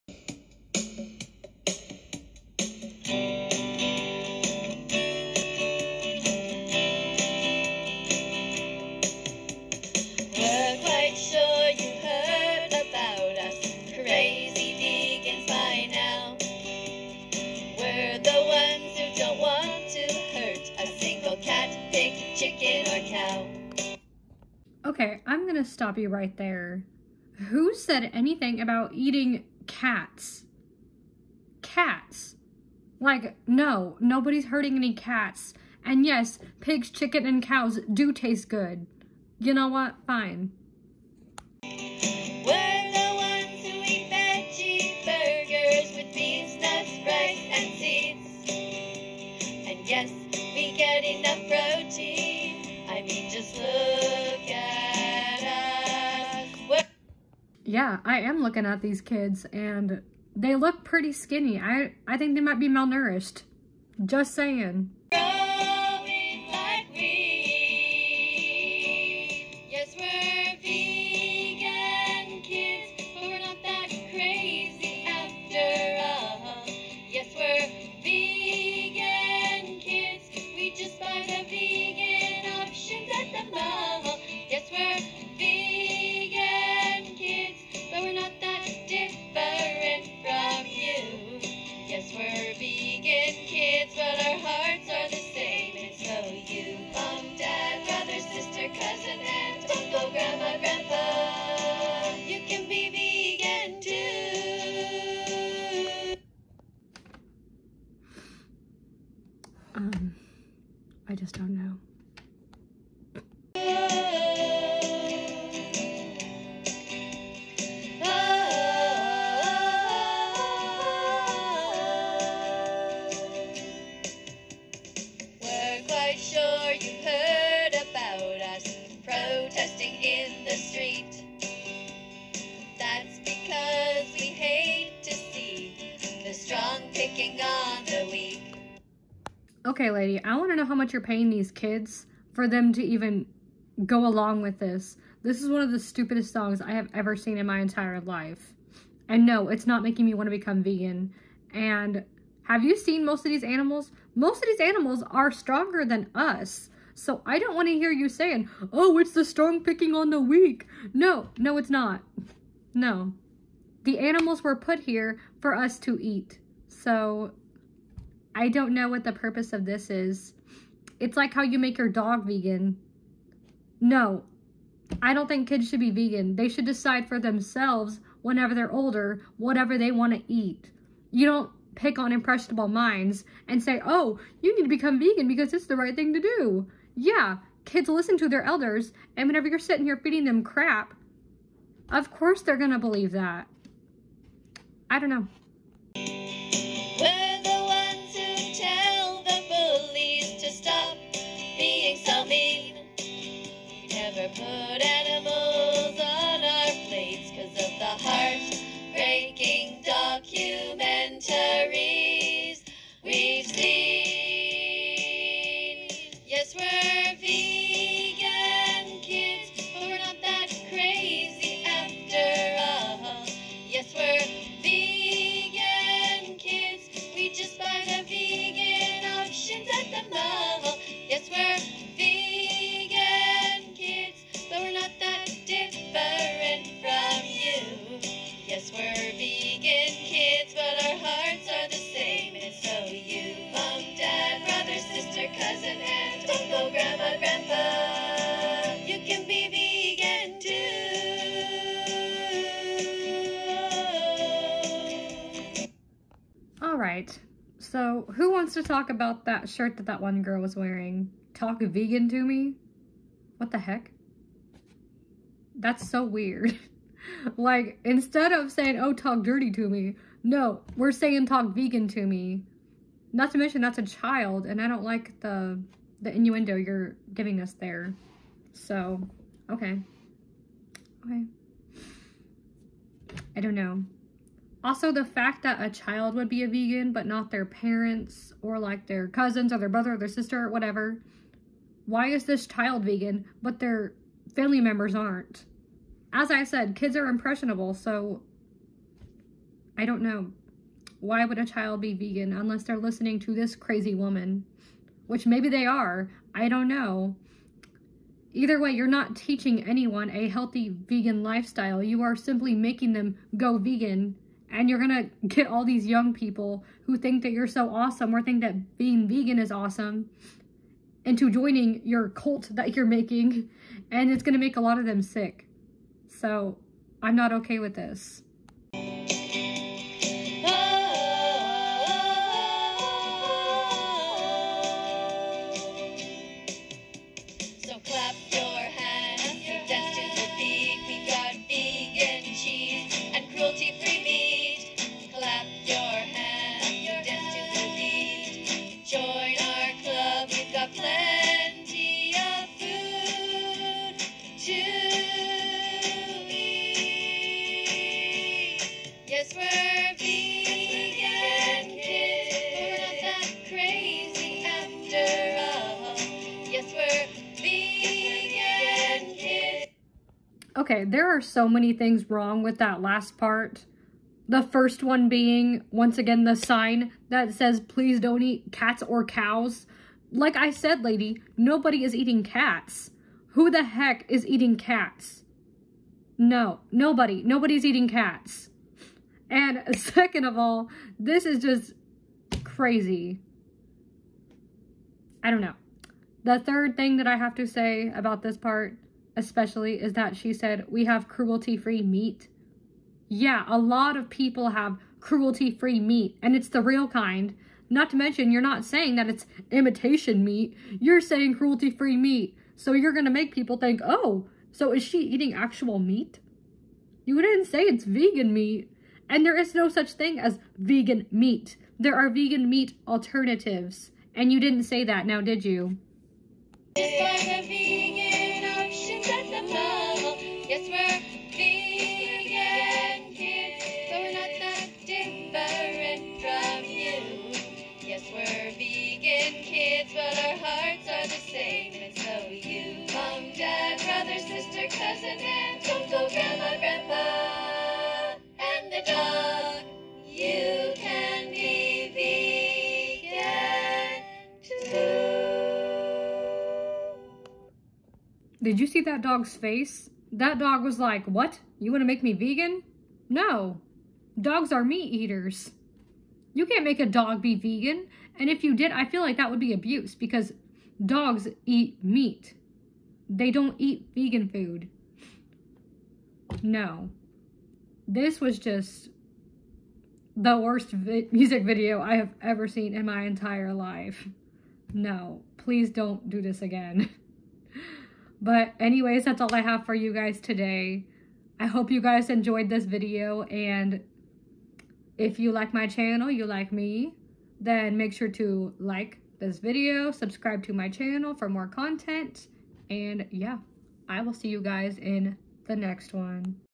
That vegan teacher is trying to brainwash kids now?!
Came across this song and it was super disturbing!